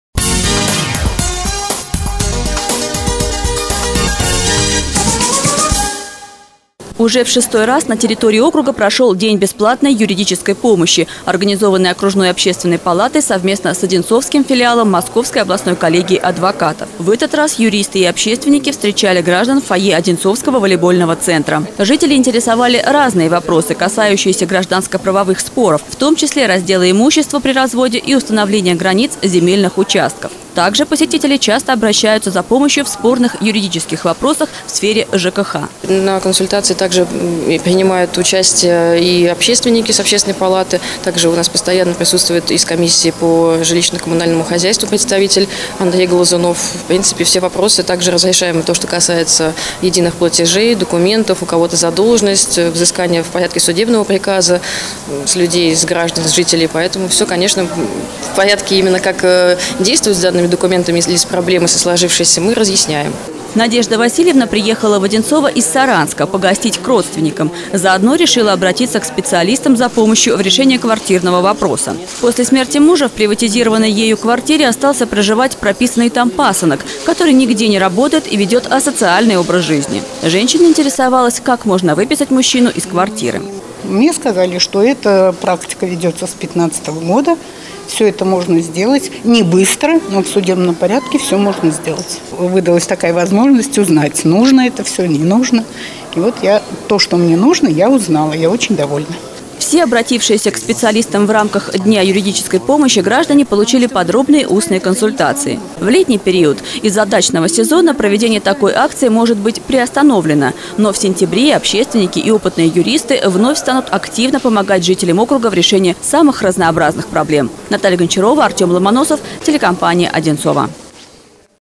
День юридической помощи
День бесплатной юридической помощи, организованный окружной Общественной палатой совместно с Одинцовским филиалом Московской областной коллегии адвокатов, прошел в Волейбольном центре.